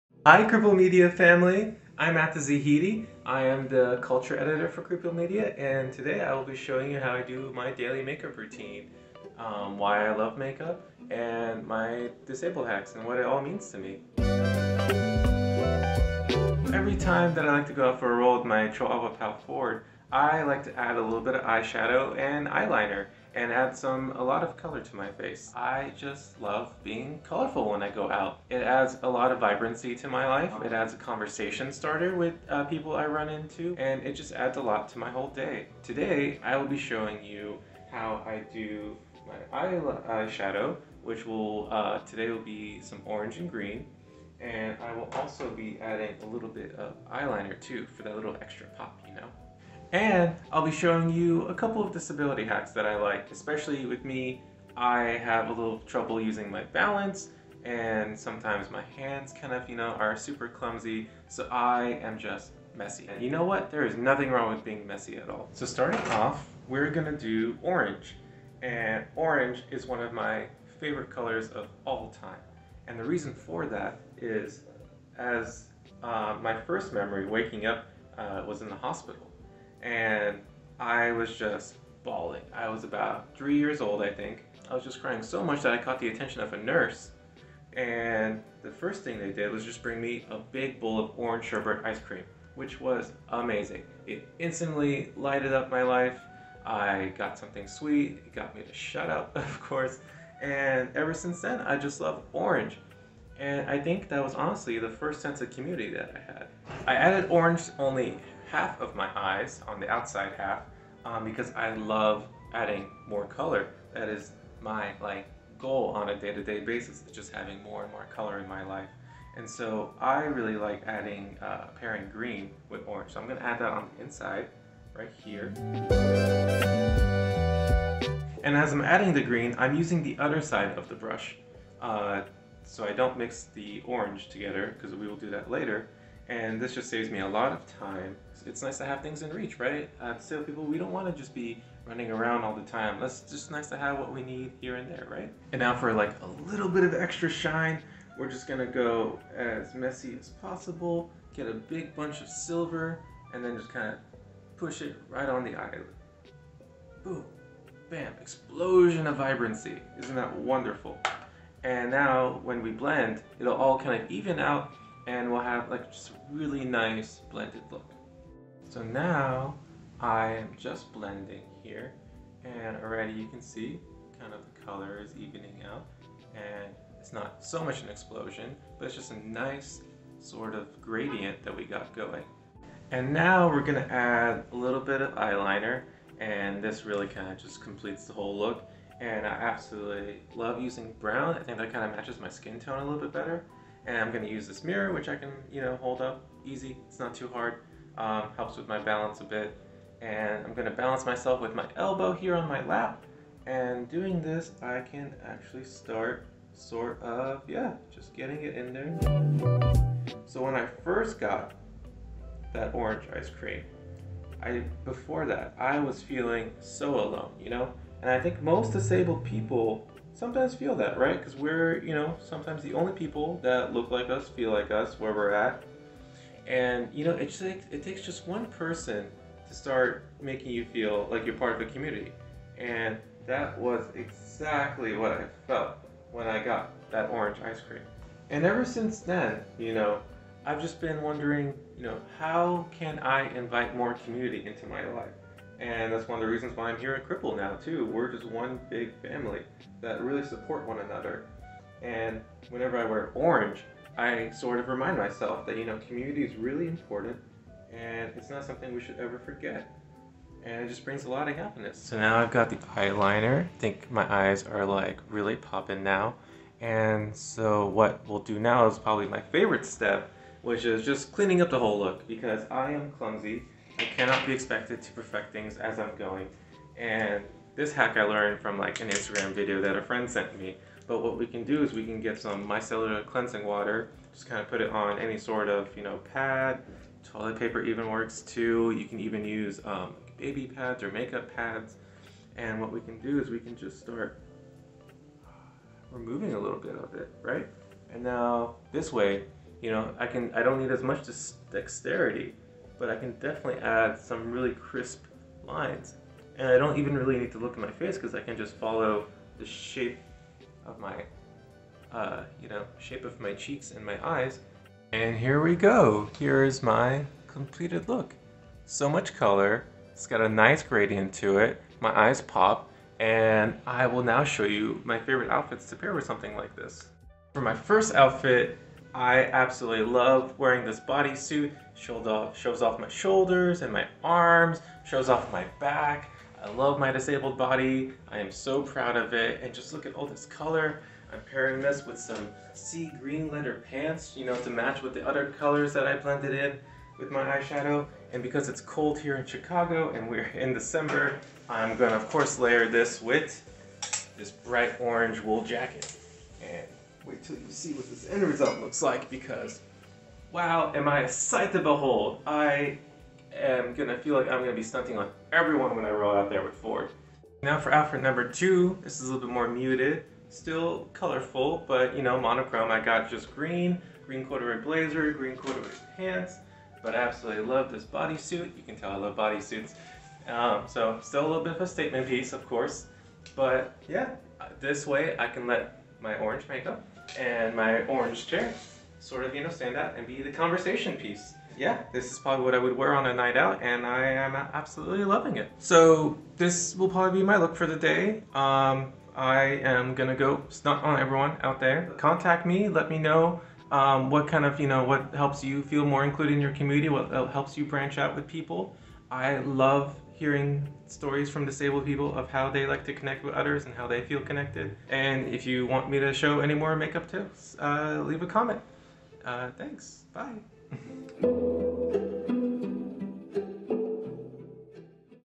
Atta Zahedi’s Guide to The Perfect Going-Out Makeup Look
In the second episode of our exclusive beauty series, we had the honor to follow our culture editor, Atta Zahedi, and peak into his favorite makeup look for going out for the day. Zahedi discusses what his favorite color, orange, means to him, how he’s been able to connect more with his community, his favorite tips for applying makeup for disabled people, and more!

Special thanks to our video editor, Raphaël Nepveu!